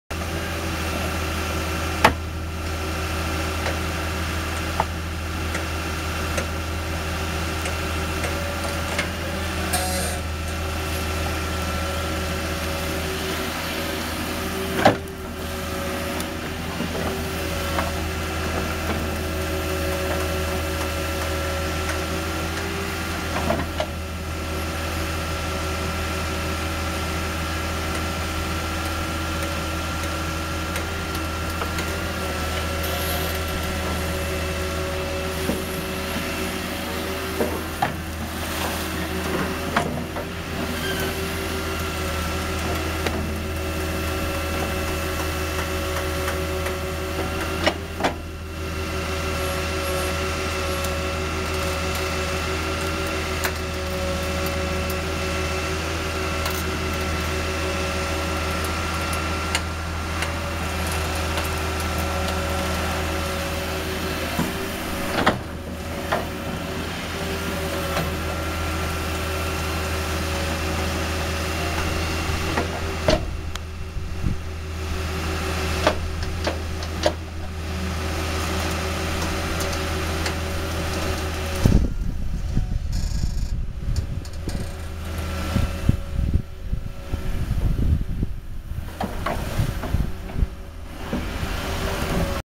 Laviamäen kosteikko, Heinola (marraskuu 2011)
Heinolan Laviamäellä avattiin umpeutuneen lahden reuna-alueille helminauhamaisesti avovesialueita. Videolla näkyy myös pohjaan asetettu maisema- ja kuivattelukivi.